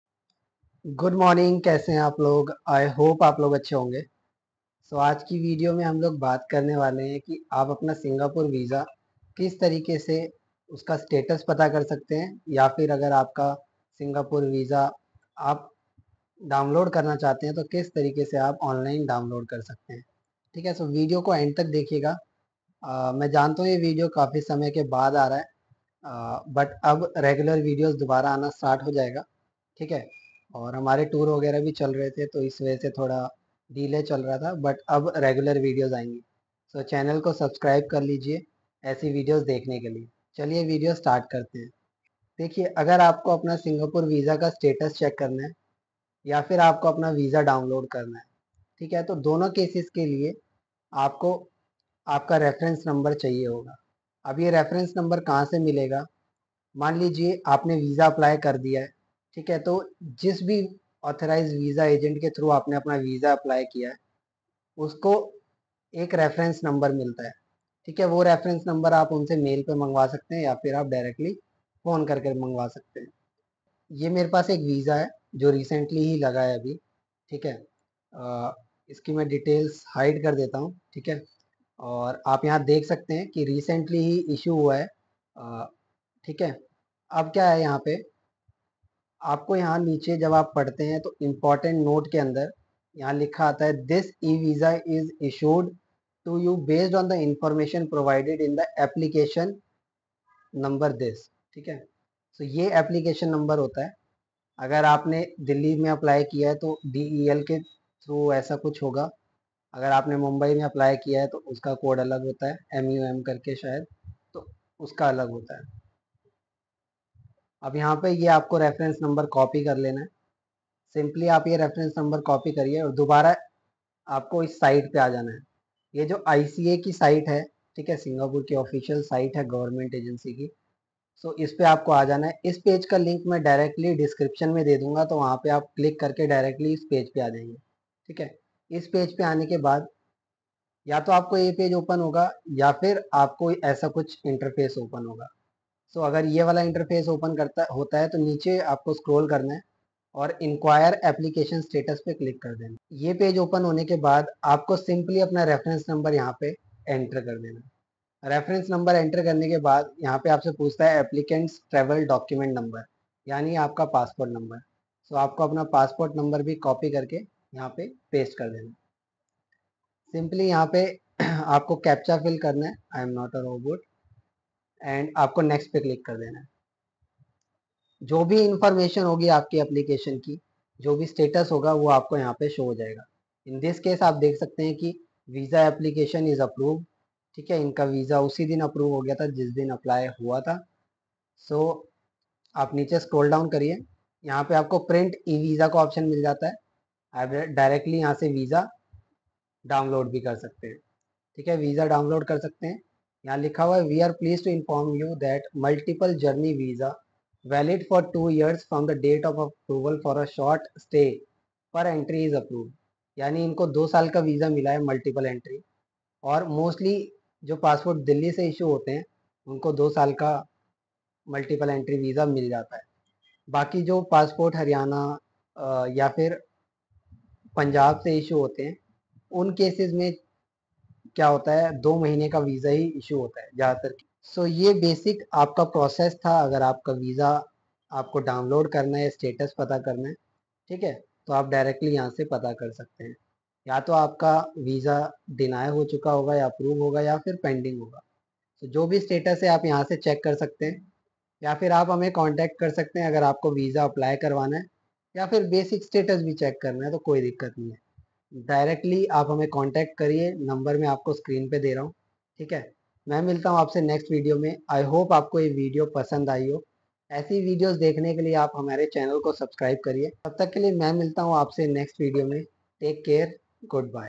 Ultimate Guide: How to Track Singapore Visa Application Status Online | ICA Step-by-Step Tutorial
Ultimate Guide: How to Track Singapore Visa Application Status Online | ICA Step-by-Step Tutorial | VFS Global

Are you eagerly awaiting the status of your Singapore visa application? Look no further! In this comprehensive tutorial, we walk you through the step-by-step process of tracking your Singapore visa application using ICA official Singapore website. Whether you're applying for a Singapore visa or eagerly waiting for an update, this video covers everything you need to know.

🔍 Key Points Covered:
- How to track visa application in VFS Global
- Tracking Singapore visa application in ICA Singapore Website
- Step-by-step guide to track Singapore visa application
- Checking Singapore visa status online
- Using Reference Number to track Singapore visa or download your visa
- Tracing Singapore e visa application status online
- VFS Global's role in Singapore visa applications

Ensure you stay informed and reduce the anxiety of waiting for your Singapore visa. Watch the video now for a stress-free visa tracking experience!